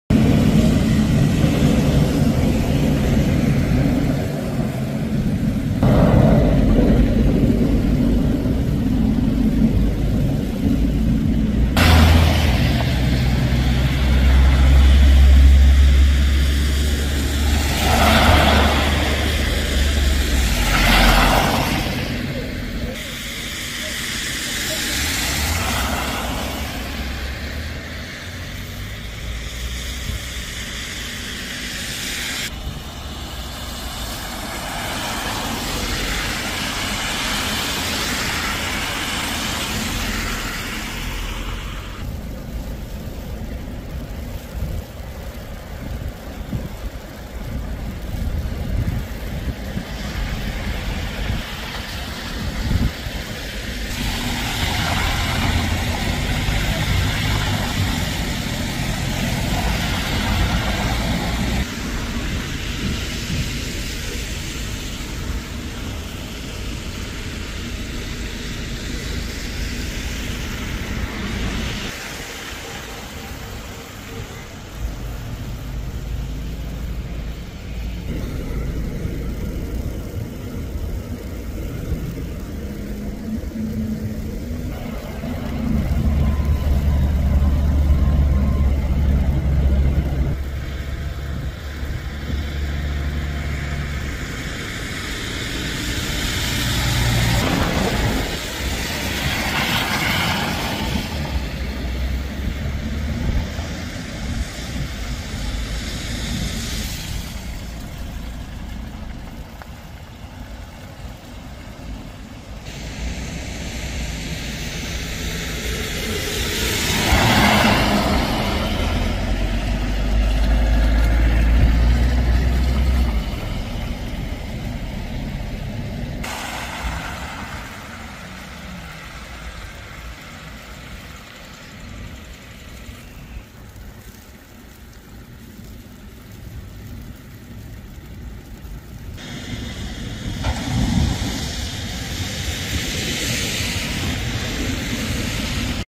Qafë Thana nën “pushtetin” e borës, temperaturat disa gradë nën zero
Reshje të dendura bore kanë përfshirë mesditën e së shtunës dhe aksin rrugor Prrenjas-Pogradec, dhe pikërisht në zonën e Qafë Thanës.